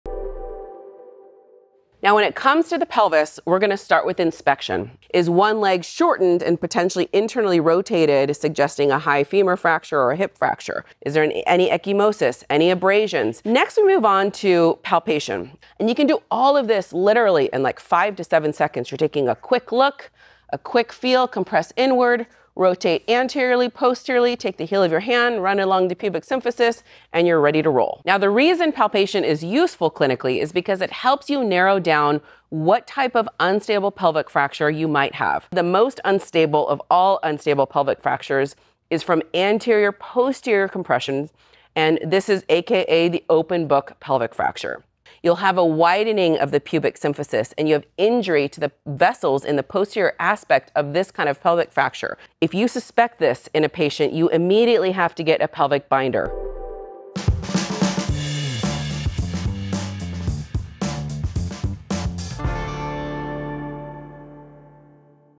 Trauma Fundamentals: Secondary Survey: Pelvis
Did you say you wanted more pelvic fracture information? 🚑‼️
We thought so. This clip from our “Trauma Fundamentals” shows proper binder placement. Did you know that in some older studies, just about half of the pelvic binders were placed incorrectly, with most of the errors being a bit too high? You have to aim for the greater trochanter to provide the best chance for your patients. 👍🏻